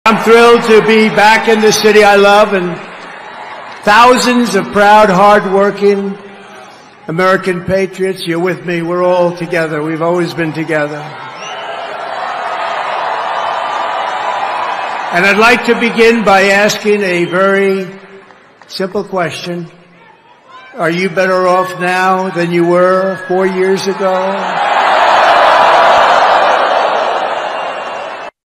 Donald Trump in NYC I'm thrilled to be back in the city I love and I'd like to begin by asking a ve
Donald Trump in NYC. I'm thrilled to be back in the city I love and I'd like to begin by asking a very simple question are you better off now than you were four years ago? NOPE! Oct 27, 2024. We were better off when Trump was in. We need somebody to take command. How's Bidenomics Now Kamalanomics, working for you? Kamala Harris = Lie, Deny, Deflect, Lie, and Blame Republicans.